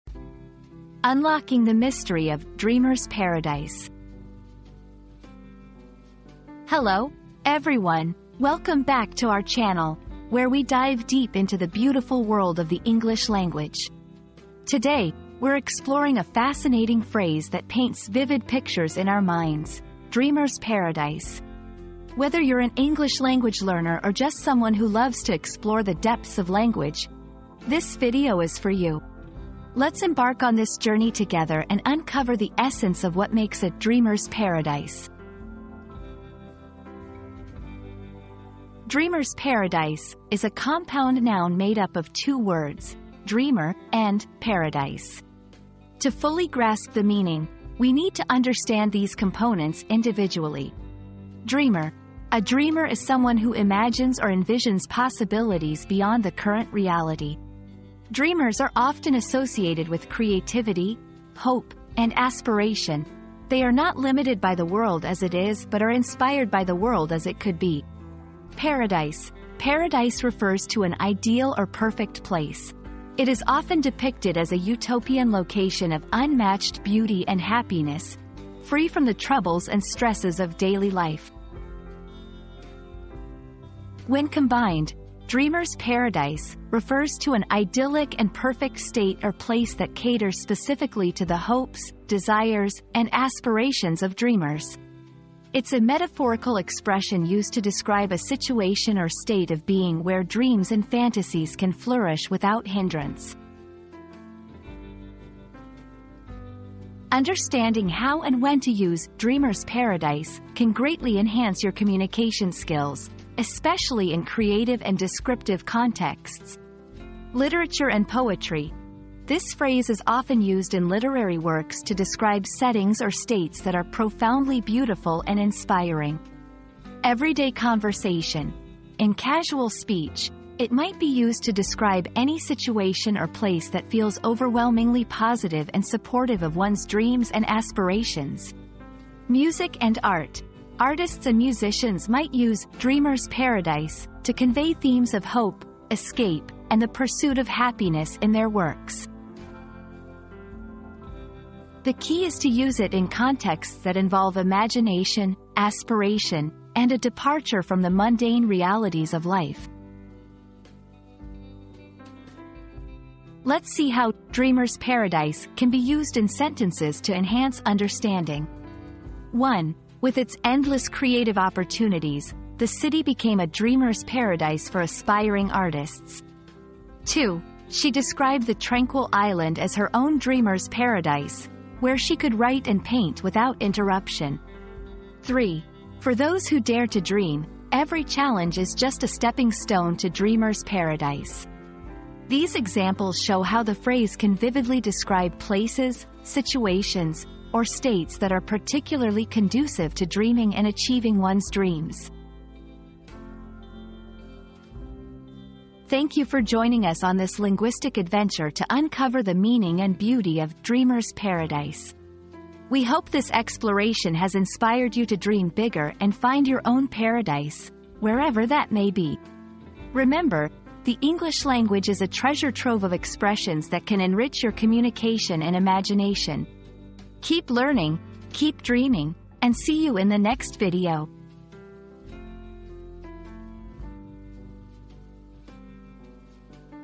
Unlocking the Mystery of "Dreamer's Paradise"
Unlocking the Mystery of 'Dreamer's Paradise' • Embark on a thrilling journey as we unravel the secrets and hidden wonders of 'Dreamer's Paradise', a place where dreams come to life. Discover the enchanting landscapes, captivating stories, and surreal experiences that await in this extraordinary realm. Get ready to be spellbound!

00:00 • Introduction - Unlocking the Mystery of "Dreamer's Paradise"
00:38 • Breaking Down the Phrase
01:46 • Usage and Context
02:42 • Examples in Sentences